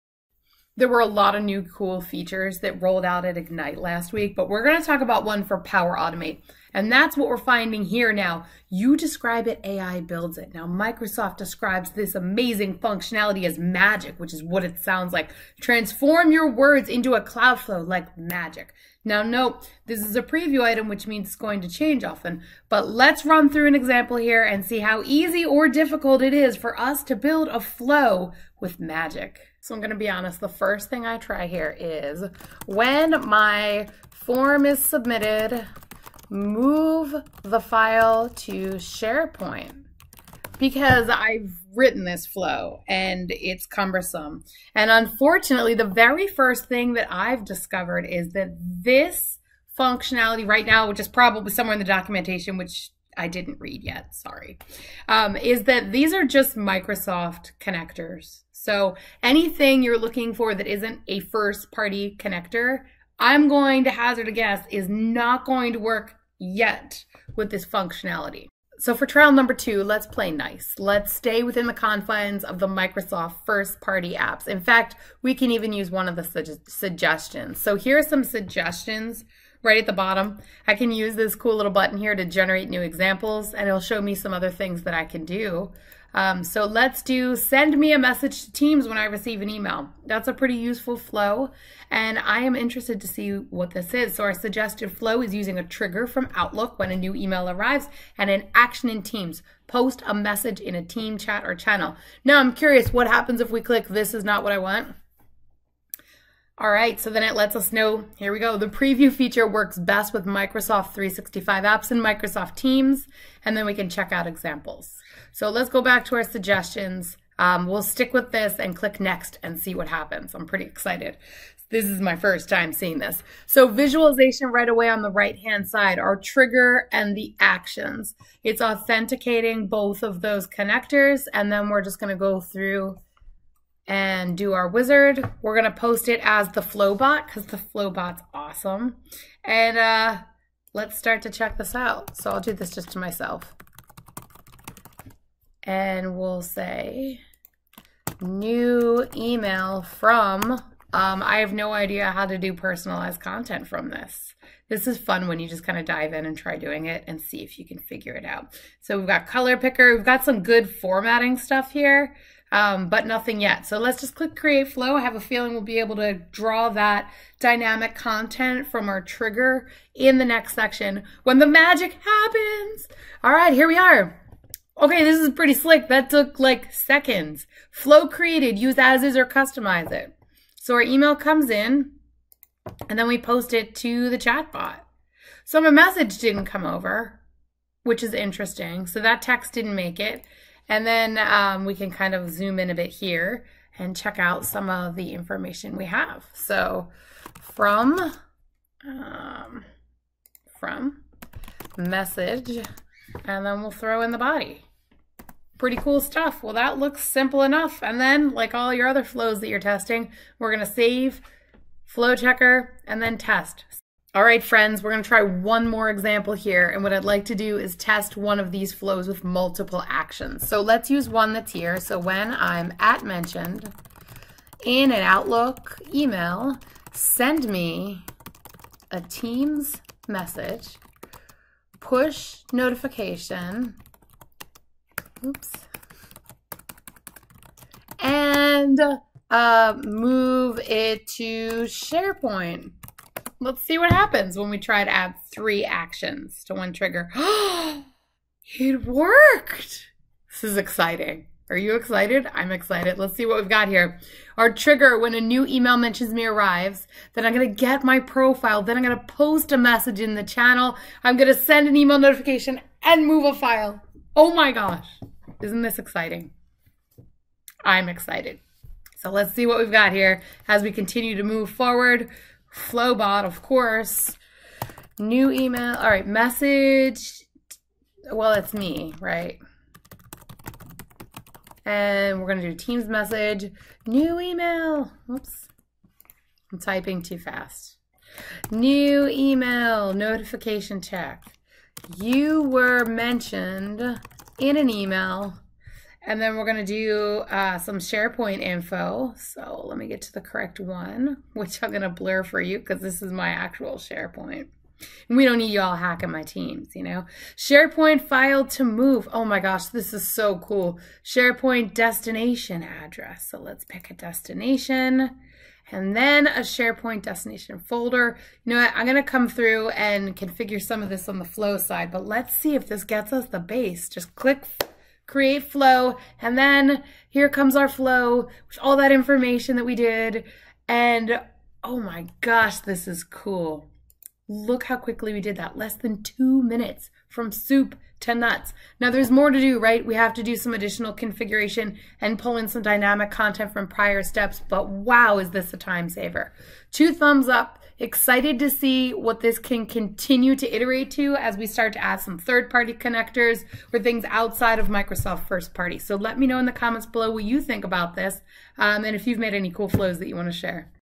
Power Automate: Describe it to design it
Transform your words into a cloud flow... like MAGIC! A preview features.
0:00 Intro
0:30 First test: Fail (not a first party connector)
1:09 Second test: Microsoft First-Party Connectors
3:34 Test with many actions